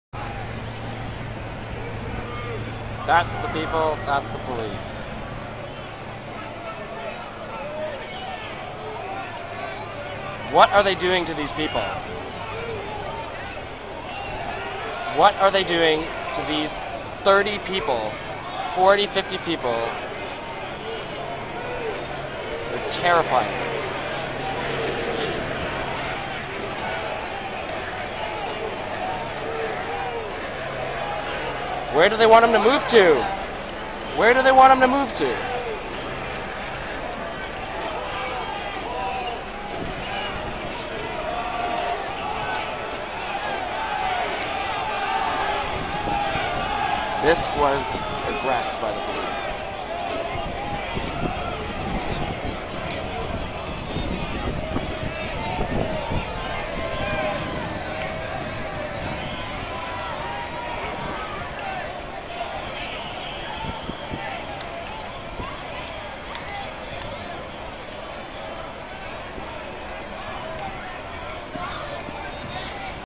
G20 Toronto - June 27, 2010 Cops Try to Incite Violence by Surrounding Peaceful Protestors
G20 Toronto June 27, 2010

Police try to surround and blockaid peaceful protestors in a corner